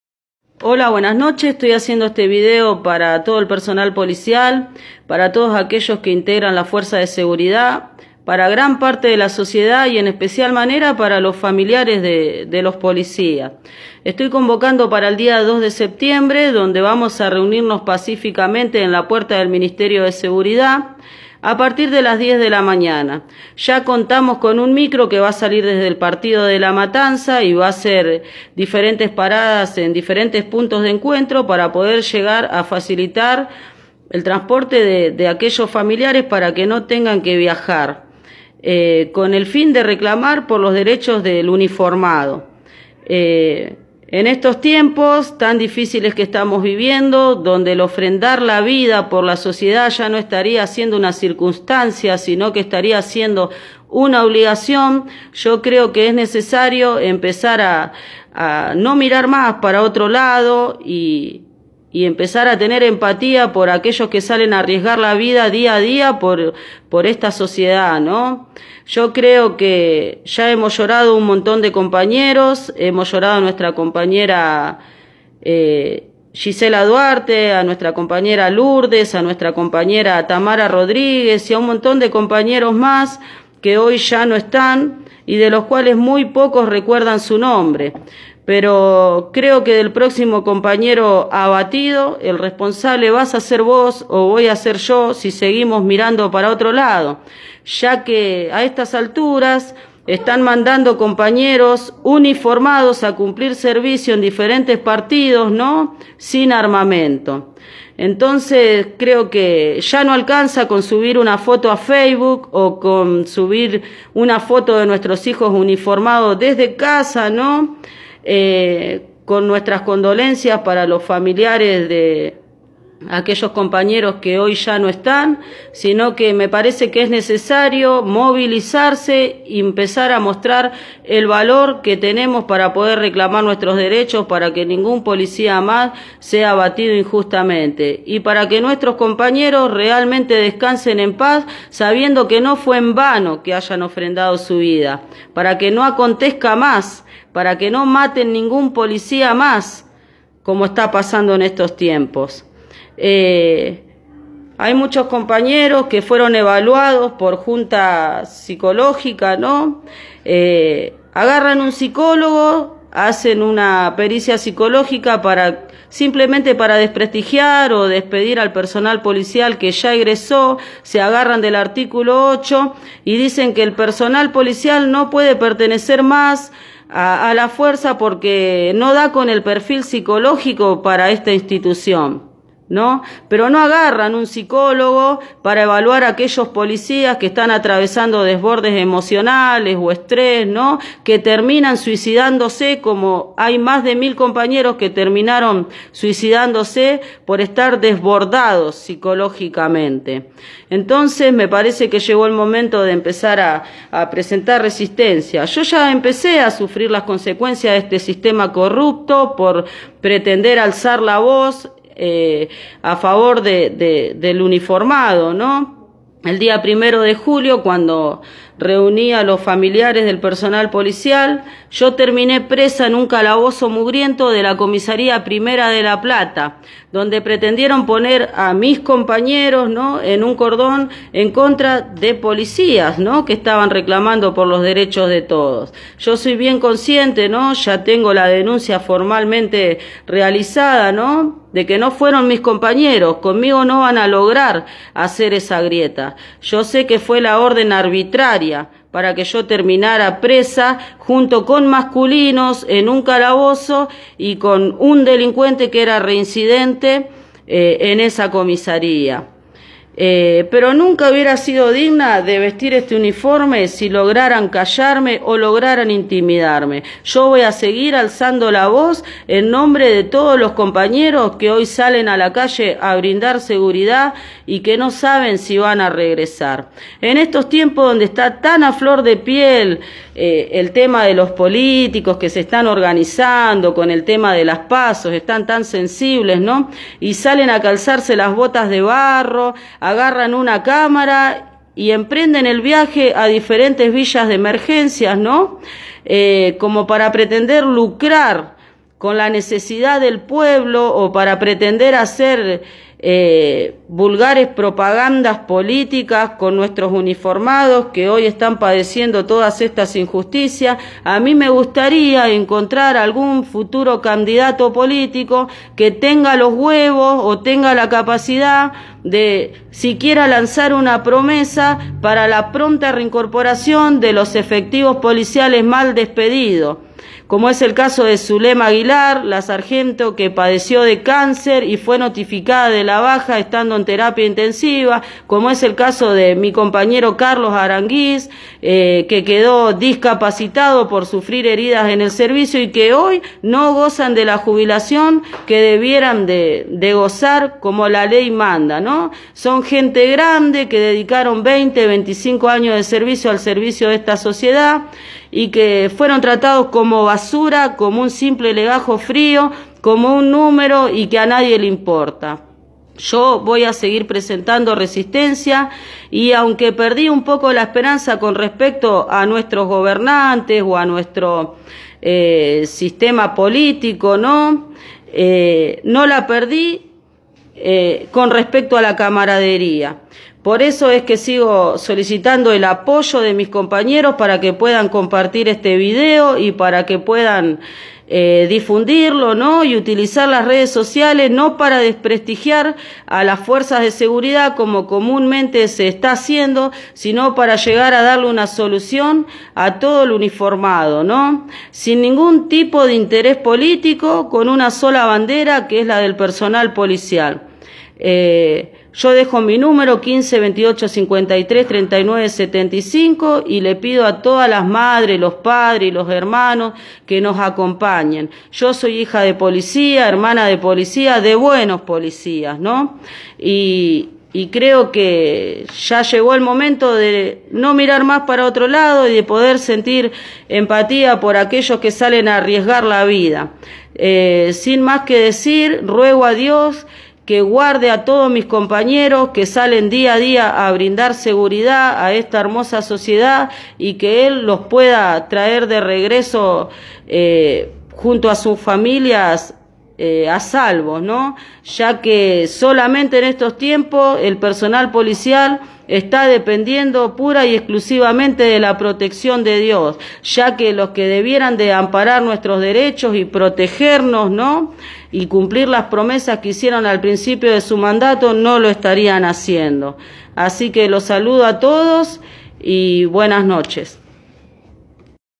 Viviana Almirón, denuncia a las autoridades del Ministerio de Seguridad
Efectivo policial, denuncia que son expuestos a patrullar desprovistos de armamento, con lo que su vida para el Ministerio de Seguridad que conduce Cristian Ritondo no vale nada.

Convoca a familiares, efectivos, vecinos y amigos a participar de un reclamo en las puertas del Ministerio de Seguridad Bonaerense en la ciudad de La Plata el día 2 de septiembre.

Nos movilizamos para que jubilen a los efectivos que despidieron por enfermarse de cáncer y los que quedaron discapacitados por enfrentamiento y que les dieron la baja en vez de jubilarlos.

Como a los compañeros que dejaron sin trabajo sin sueldo y sin obra social por tener hijos discapacitados.

Por los que están sacando a trabajar uniformados SIN ARMAS, sólo con una tonfa.